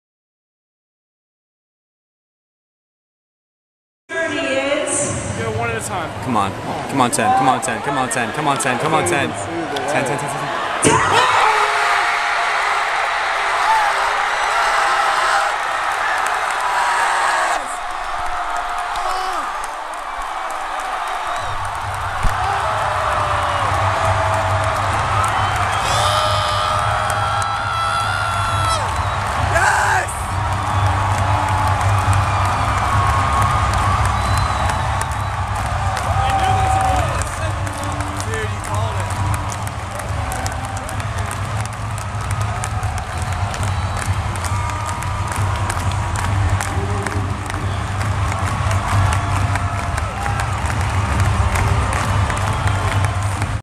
THON 2012 Reveal Reaction
A POV reaction to the total reveal for THON 2012. Penn State students helped raise over 10.6 million dollars to help children fighting pediatric cancer.